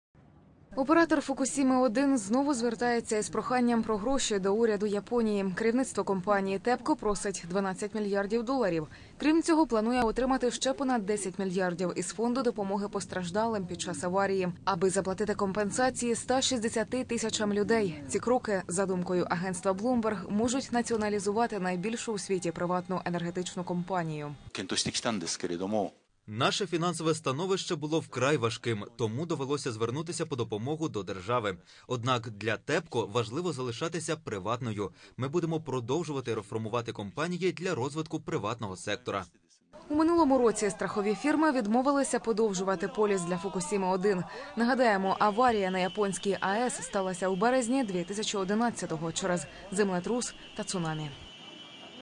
На Фукусіму-1 просять ще 22 млрд доларів
ubr.ua - украинский бизнес ресурс : Оператор "Фукусіми-1" знову звертається із проханням про гроші до уряду Японії. Керівництво компанії ТЕПКО просить 12 мільярдів доларів. Крім цього, планує отримати ще понад 10 мільярдів із фонду допомоги постраждалим під час аварії, аби заплатити компенсації 160 тисячам людей. Ці кроки, за думкою агентства Блумберг, можуть націоналізувати найбільшу у світі приватну енергетичну компанію.
Тошіо Нішізава, президент ТEPCO: "Наше фінансове становище було вкрай важким, тому довелося звернутися по допомогу до держави. Однак для TEПКO важливо залишатися приватною. Ми будемо продовжувати реформувати компанії для розвитку приватного сектора".
У минулому році страхові фірми відмовилися подовжувати поліс для Фукусіми-1. Нагадаємо, аварія на японській АЕС сталася у березні 2011-го через землетрус та цунамі. .